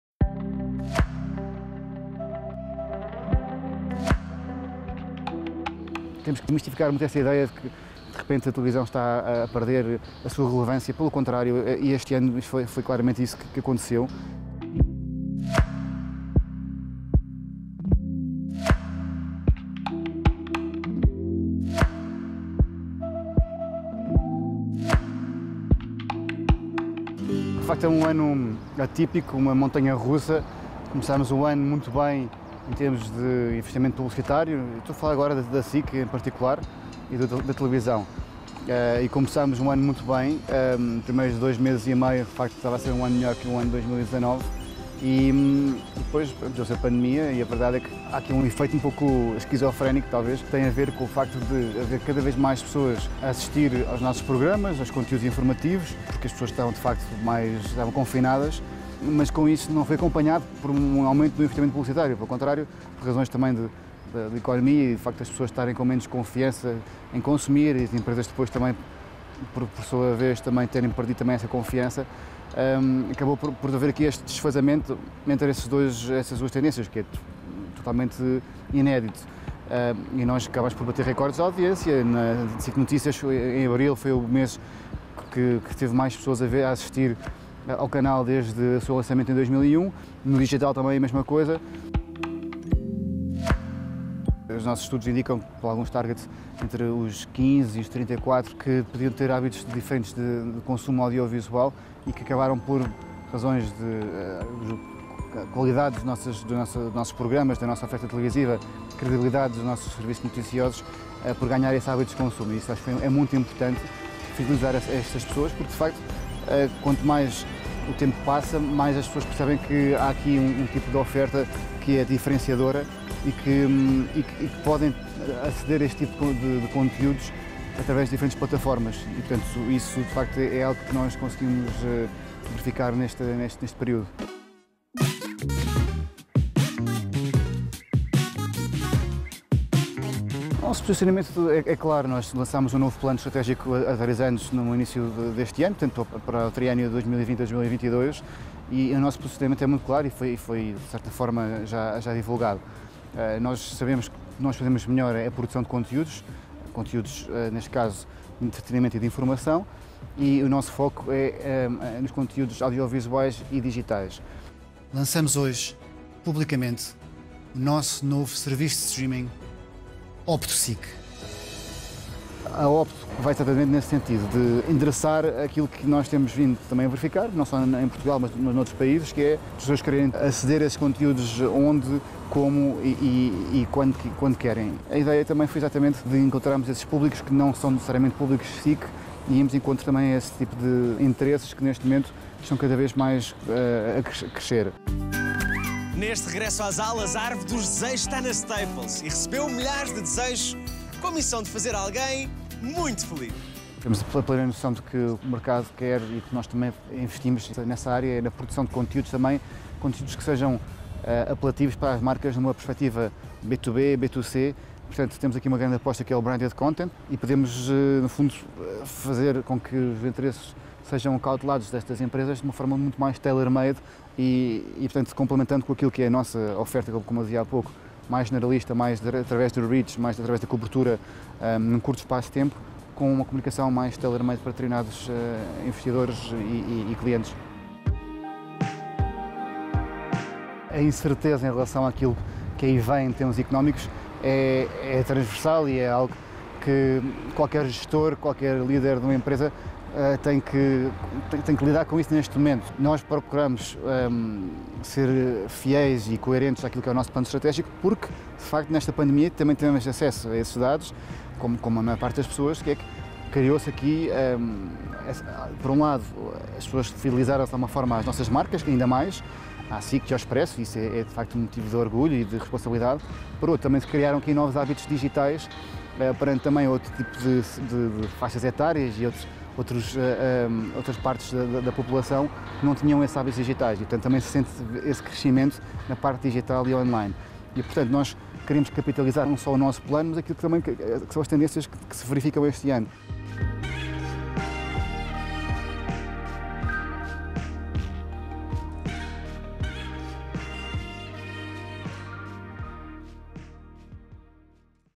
This revolution will be televised | Beyond Imagens de Marca
In this episode of Beyond Imagens de Marca series we interviewed Francisco Pedro Balsemão, the CEO of one of the biggest media groups in Portugal, Grupo Impresa. SIC is the leading channel in Portugal 20 months in a row. Television had a fundamental role during quarantine, it entertained and informed the Portuguese people during this dark time of our History. During lockdown Grupo Impresa got the loyalty of new spectators and wants to start a conversation with brands and show the market new technological solutions that will revolutionize the way we view our content.
Neste episódio da série Beyond Imagens de Marca conversamos com Francisco Pedro Balsemão, presidente executivo de um dos maiores grupos de media em Portugal, o Grupo Impresa. O canal generalista SIC que pertence ao grupo afirma a sua liderança no mercado português há 20 meses consecutivos. A televisão voltou a reafirmar-se durante a quarentena e mostrou ser mais relevante que nunca, o seu papel foi fundamental em informar e entreter os portugueses durante esta fase negra da nossa História. Durante a quarentena o grupo fidelizou novos espectadores e é assente nesse reach que quer construir um diálogo com as marcas e apresentar novas soluções ao mercado.